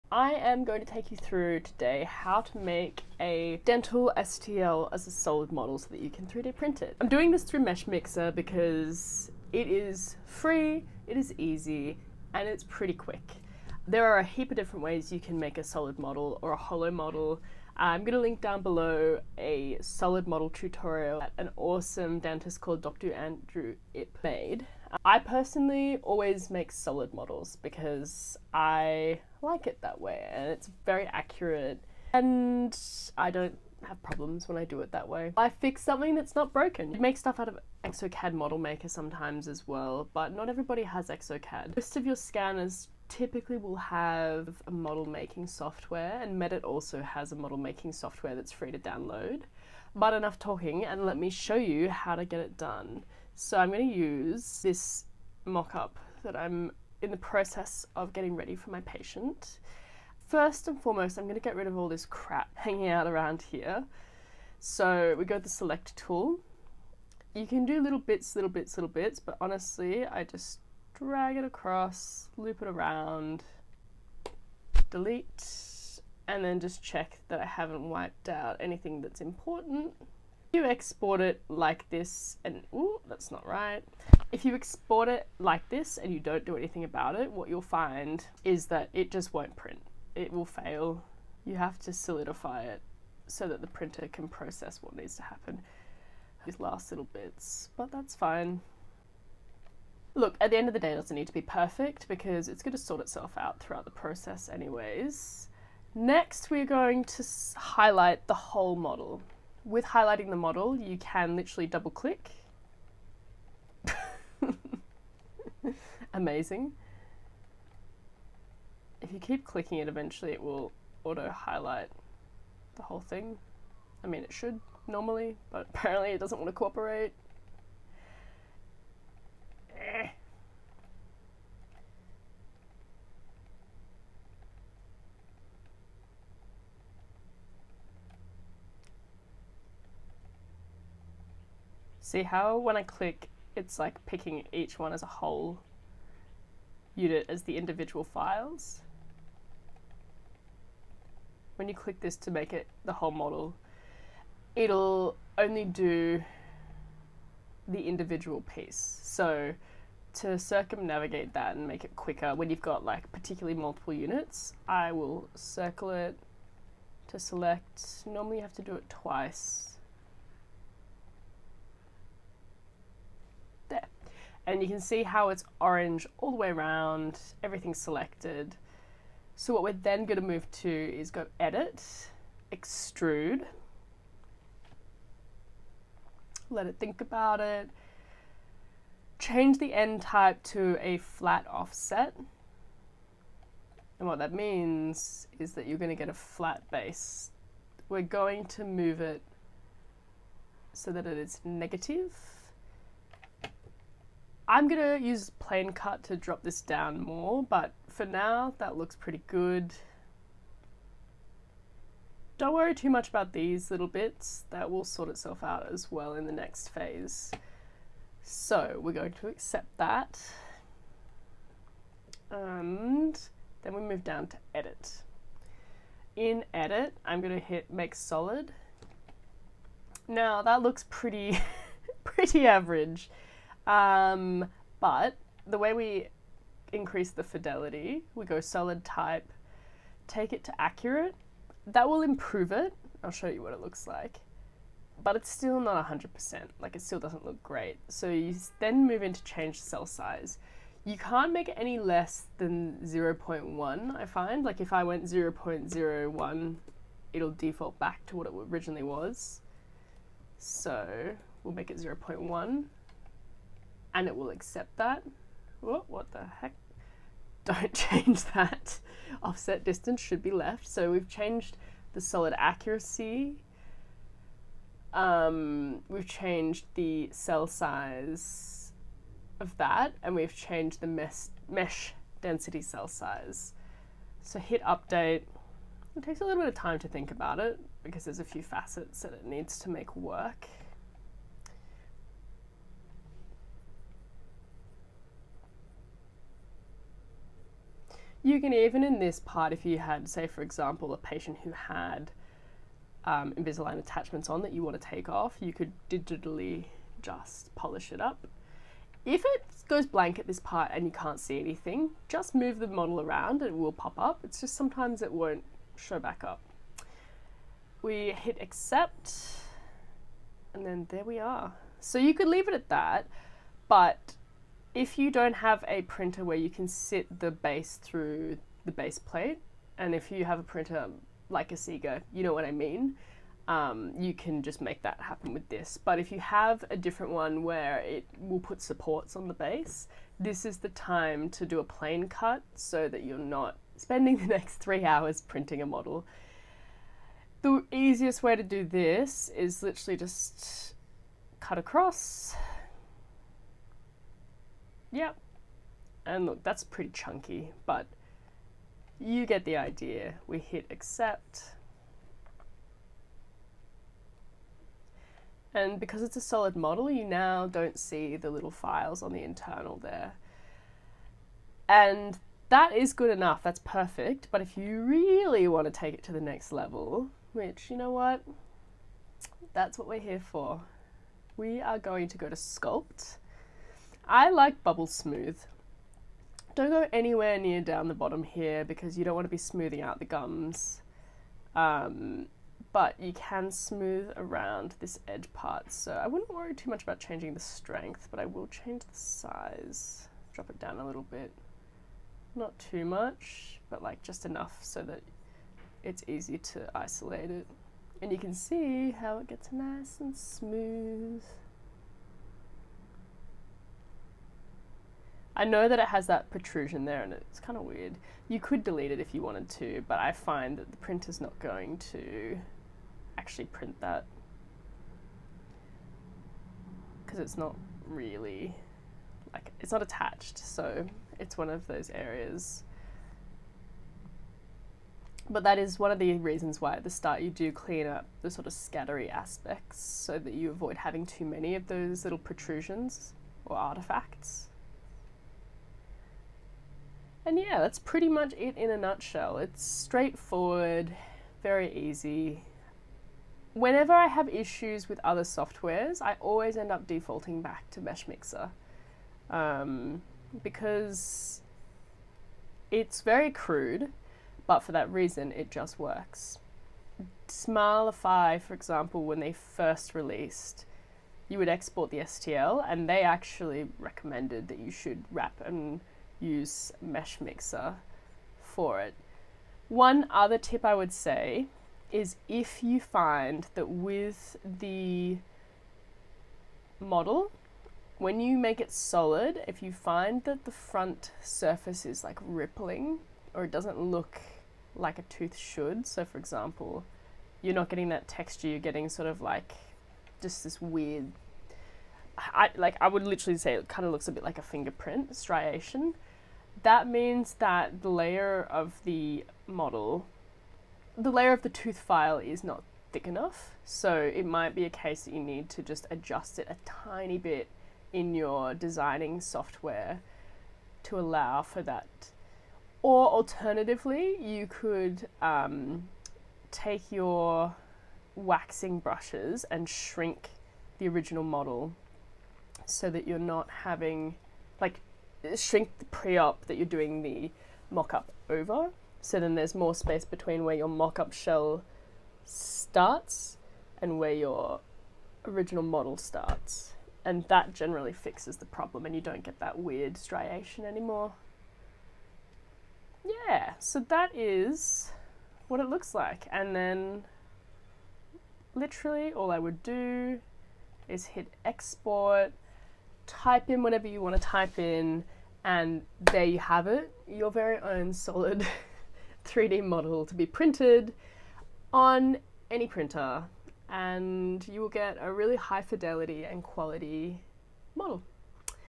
How to Make a Solid Dental Model for 3D Printing (FREE on Meshmixer)
Discover how to craft a precise dental model for 3D printing without breaking the bank. Our tutorial walks you through the entire process using Meshmixer, a completely free software. From importing scans to perfecting details, this step-by-step guide is tailored for professionals and enthusiasts alike.